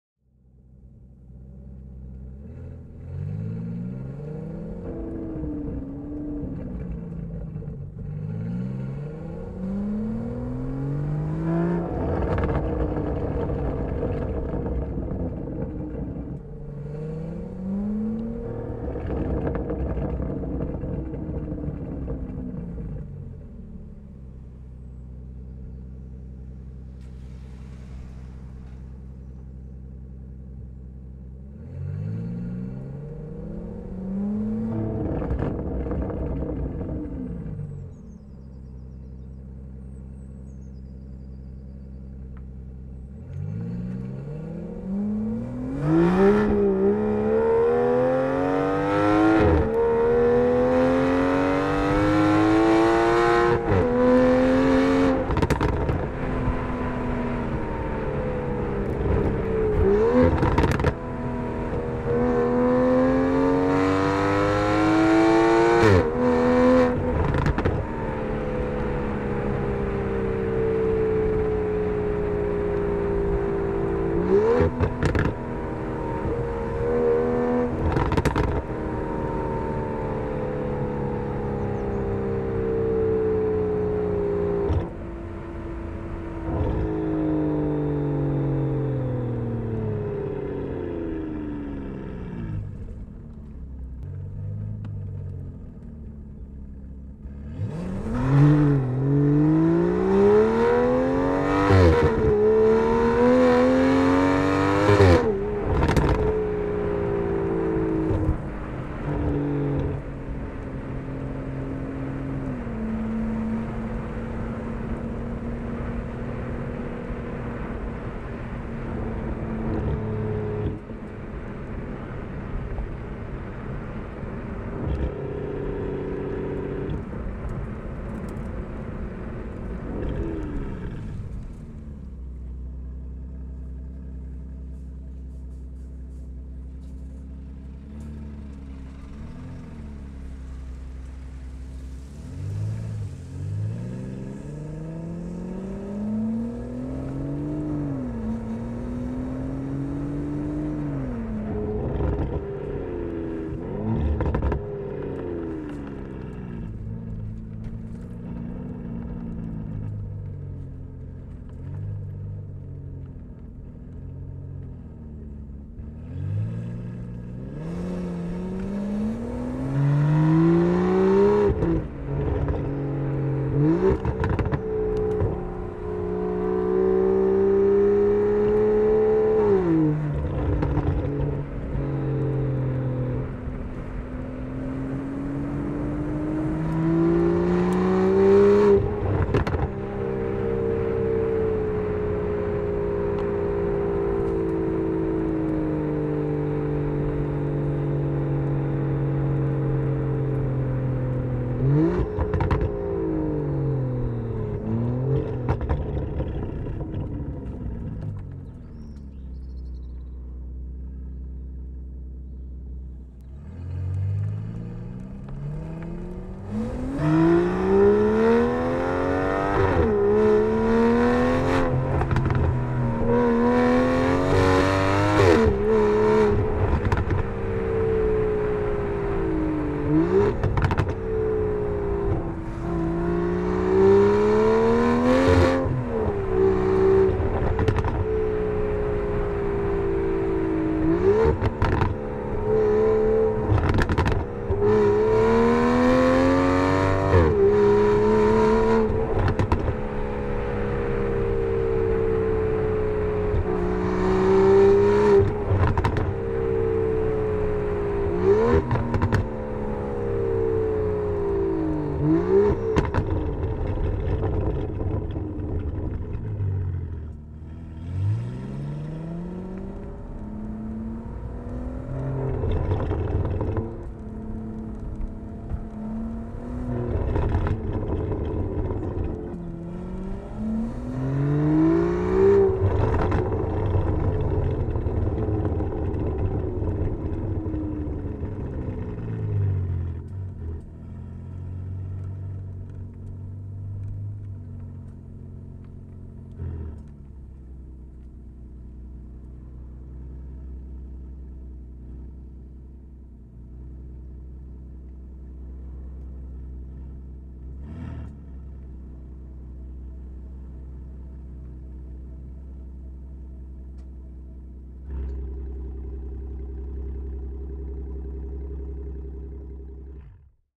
Aston Martin DBS Superleggera Interactive VR 360 Sport Exhaust Sound Experience
A fully interactive short clip where you get to ride along on the back of a Aston Martin DBS Superleggera equipped with our Ceramic Coated 'Race' Catalysts and a Tian Sport Exhaust with Sound Architect™ Active Valve Technology.